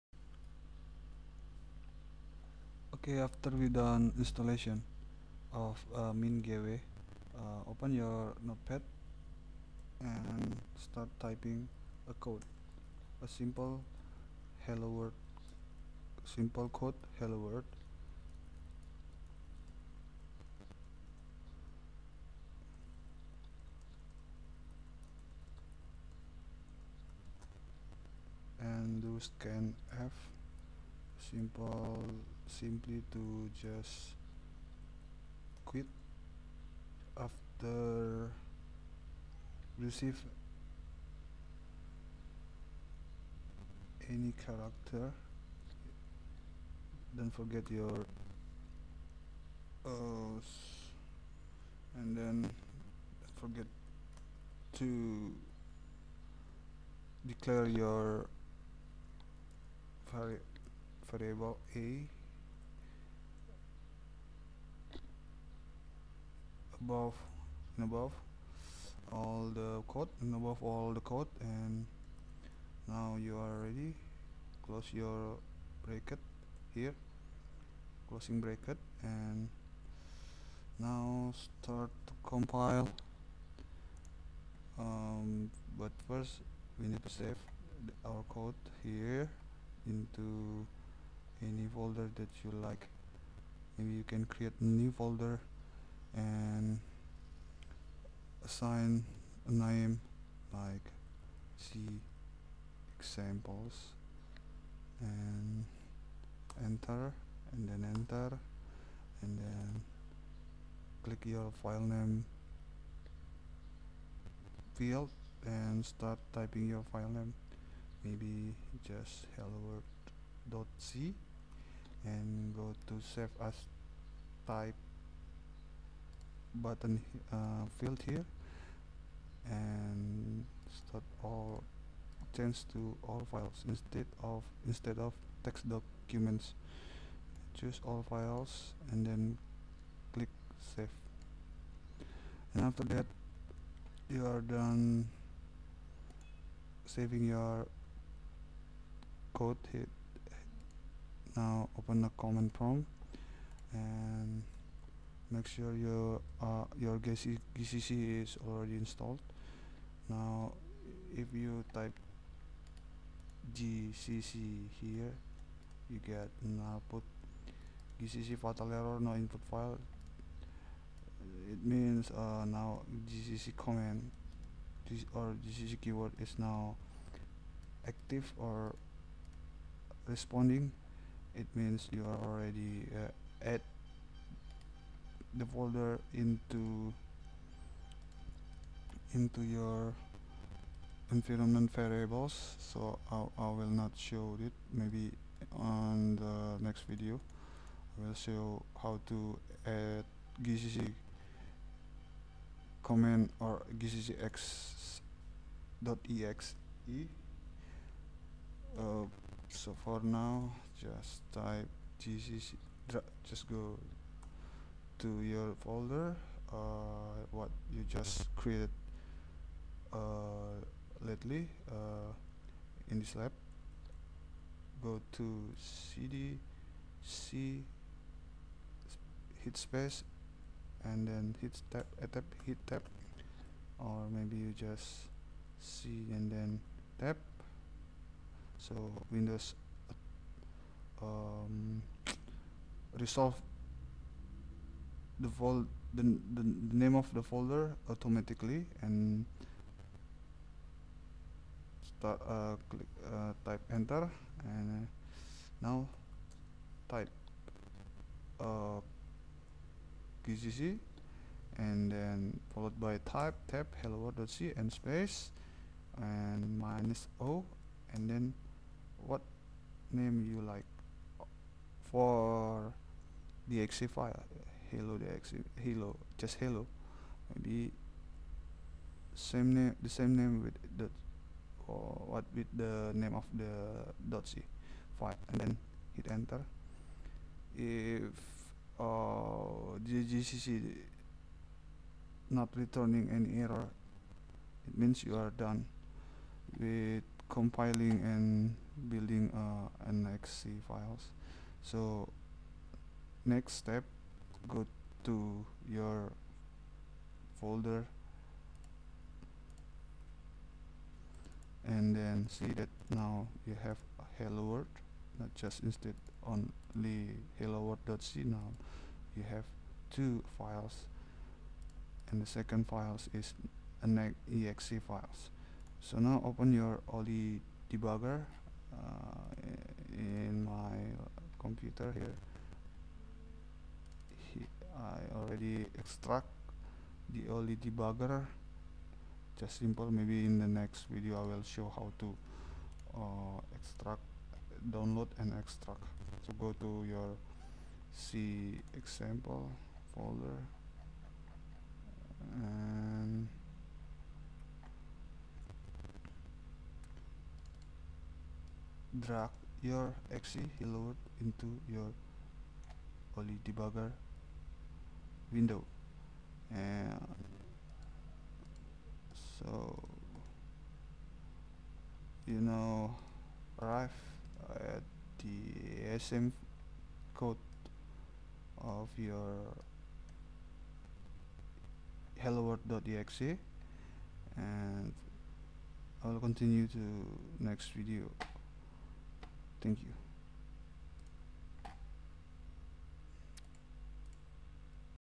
C - Hello World & .exe
This video telling you about the basic/fundamental of how to create a C source-code using notepad and compile that source-code to .exe and open/load/debug that exe file with ollydbg software to start debugging.

I use/download these softs: TDM-GCC MinGW Compiler and
Ollydbg from here:

The first step is write simple basic hello world program
The second step is to complie the program into exe file
The third step is
4th. Open Ollydbg software
5th. Drag your exe file to recently opened Ollydbg software
6th. Study your assembly code within Ollydbg software